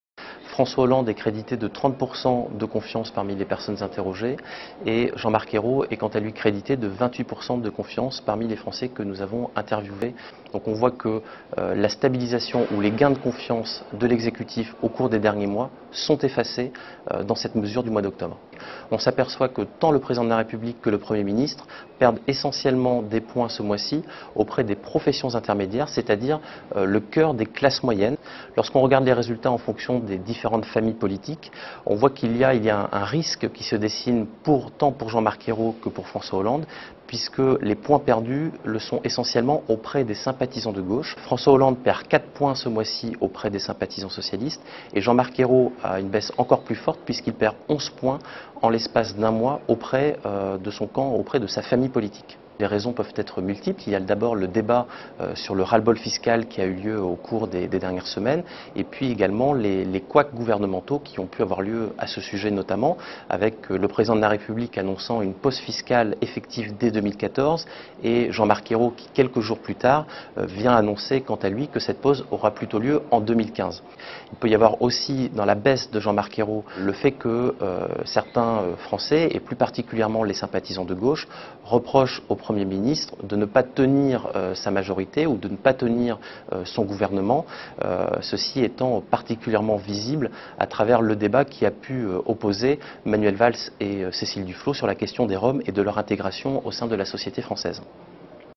Y-M Cann (CSA) : "La confiance dans l'exécutif s'érode auprès de sa propre famille politique"
Le directeur en charge de l'Opinion à l'Institut CSA livre son analyse de la nouvelle chute de confiance des français envers l'exécutif révélée par le baromètre politique CSA - "Les Echos" du mois d'octobre.